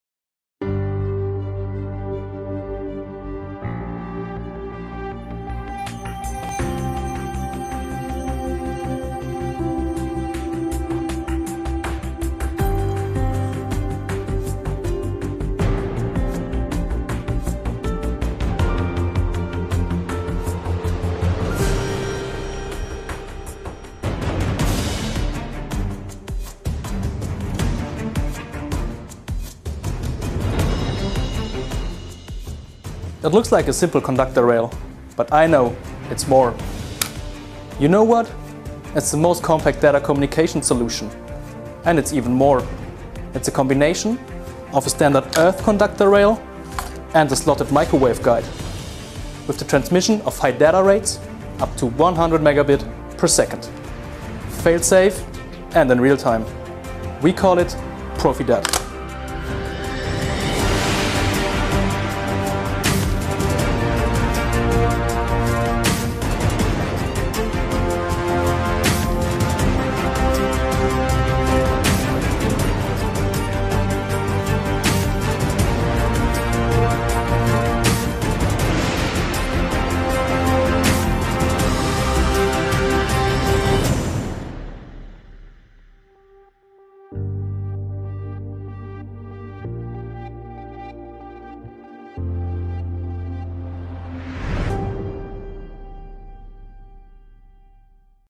Realtime Data Transmission for Cranes and Heavy Machinery with ProfiDAT
ProfiDAT® is a state of the art Data Transmission system specially designed for the harsh and highly demanding requirements on cranes or heavy machinery. The use of the ground rail for data makes it possible to convert a standard 3+PE (Earth) conductor rail configuration into a 3+PE (Earth) data solution – up to 100Mbps – without adding an additional profile.

Conductix-Wampfler is a world’s leading supplier of mobile energy supply and data transmission systems.